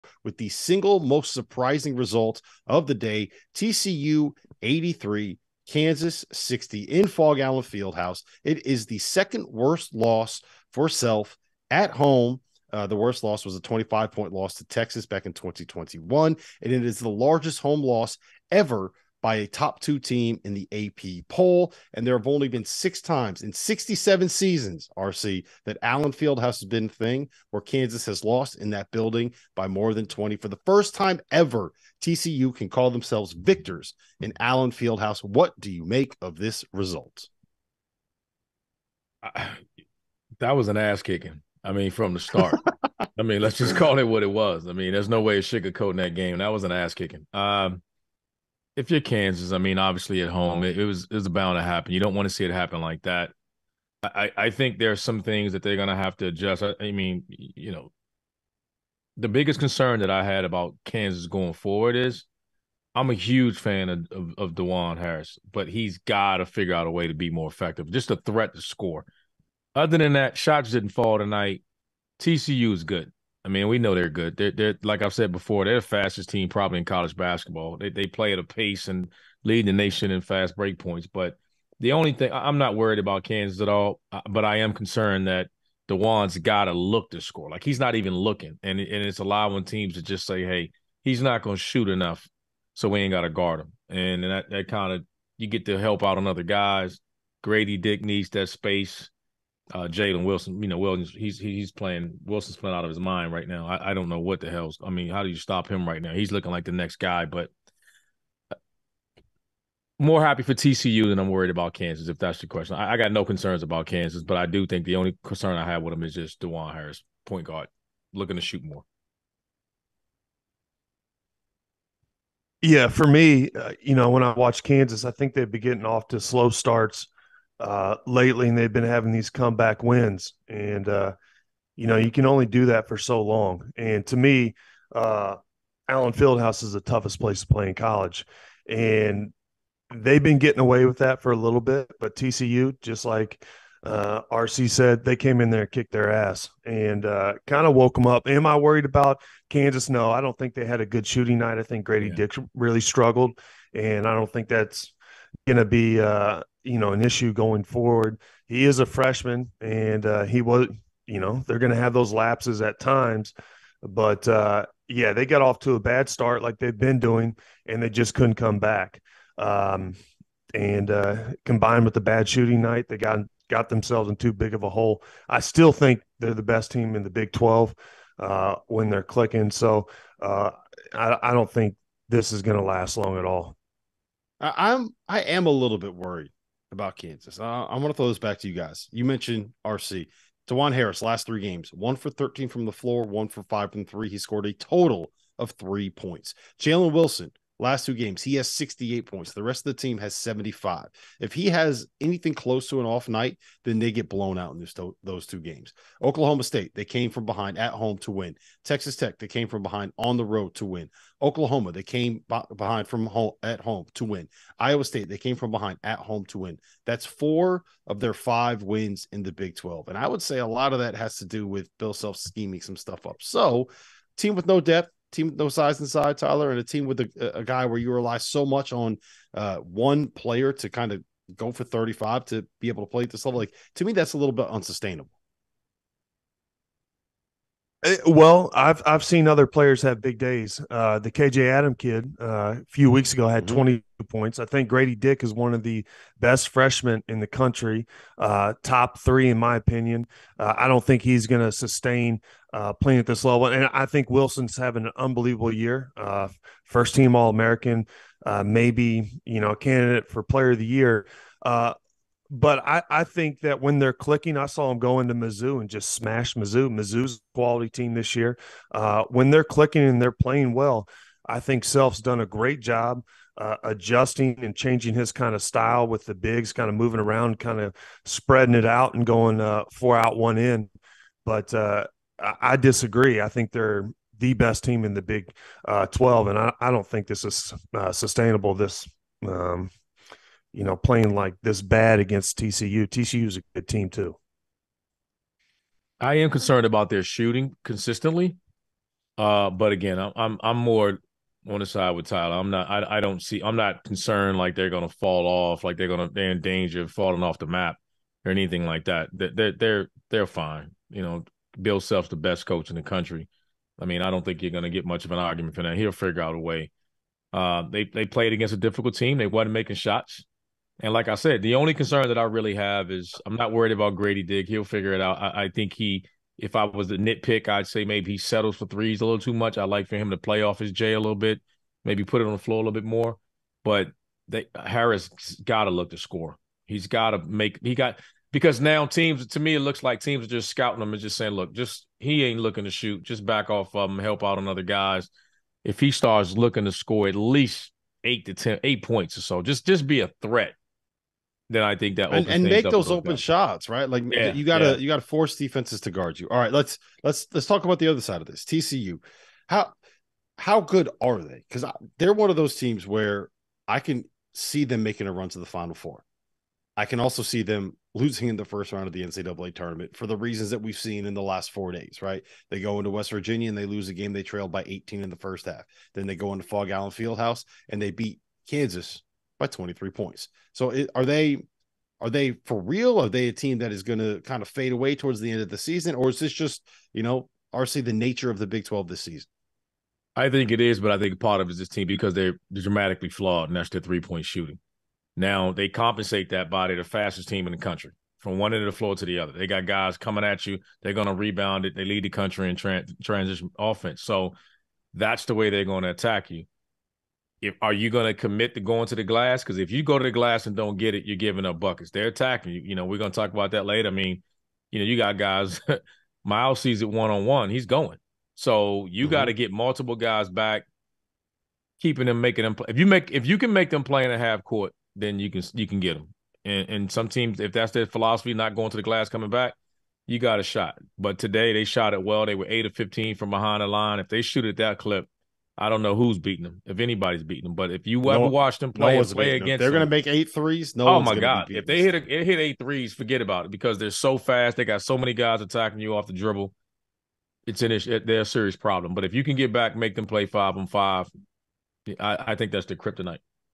INSTANT REACTION: TCU destroys Kansas!! | Jayhawks SHOCKED at home! | AFTER DARK
The Horned Frogs just handed Kansas their second-worst home loss in the Bill Self era!